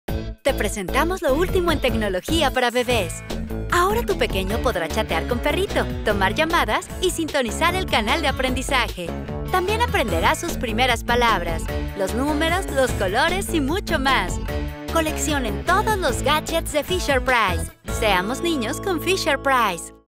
AD: Fisher-Price | Gadgets: La mejor tecnología para bebés.
Conoce la línea de Gadgets de Fisher-Price. La tecnología que tu bebé necesita para aprender sus primeras palabras, los colores, los números y mucho más.